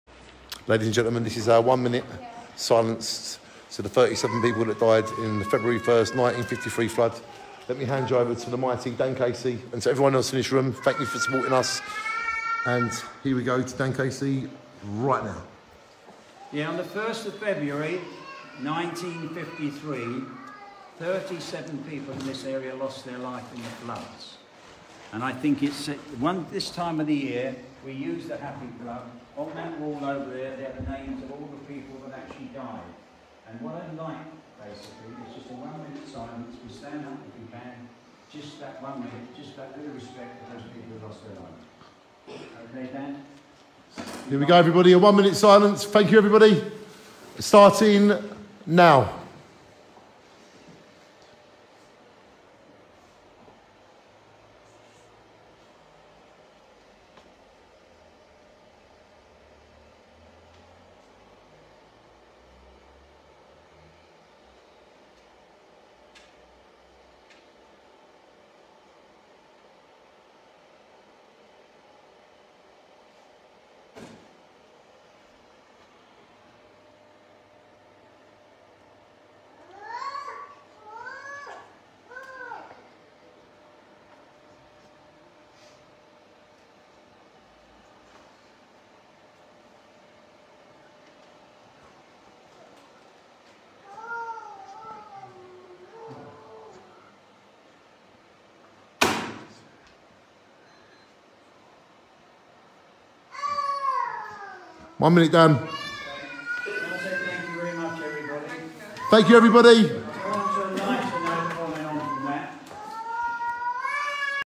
Jaywick Sands one minute silence to remember the 37 souls that lost their life’s in 1953 February 1s
Jaywick Sands one minute silence to remember the 37 souls that lost their life’s in 1953 February 1st floods with Daniel Casey at Jaywick Sands Happy Club